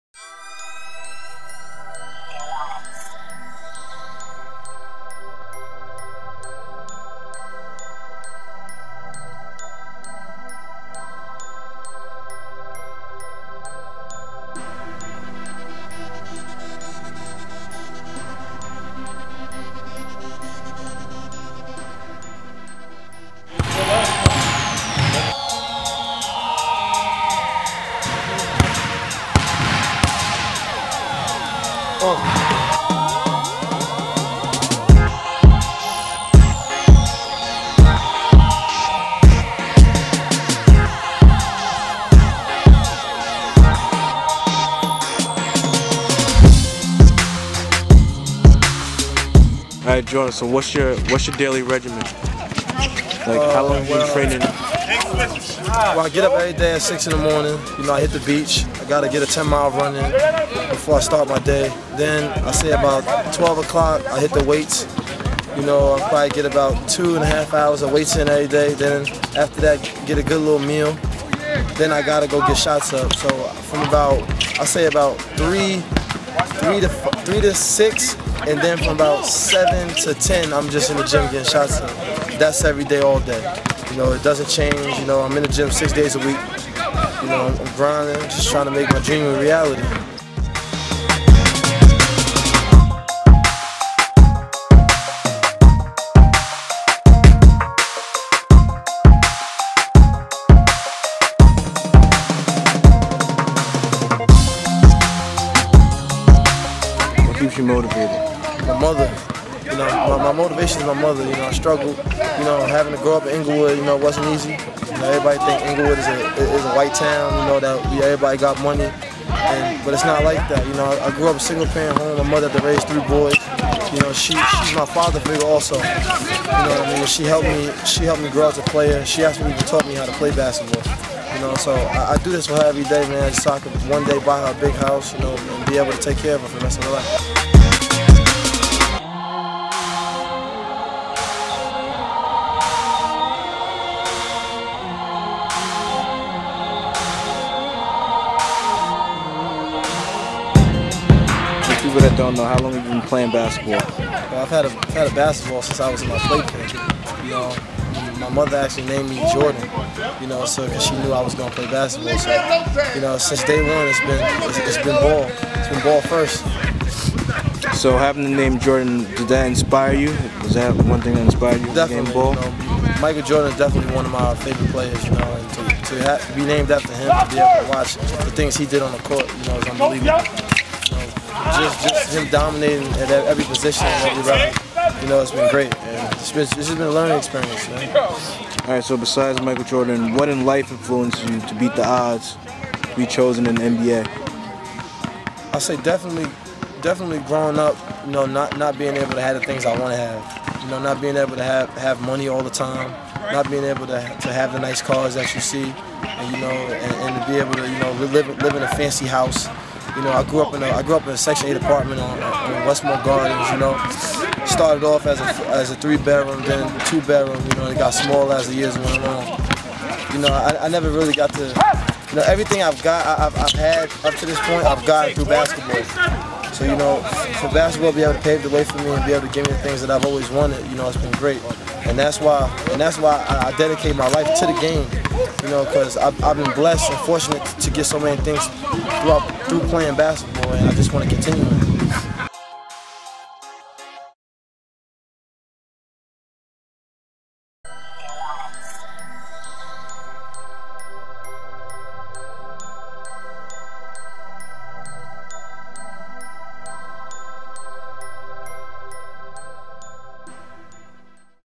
Jordan Theodore (Documentary)
Jordan Theodore working on his jump shot and chillin in McKay Park in Englewood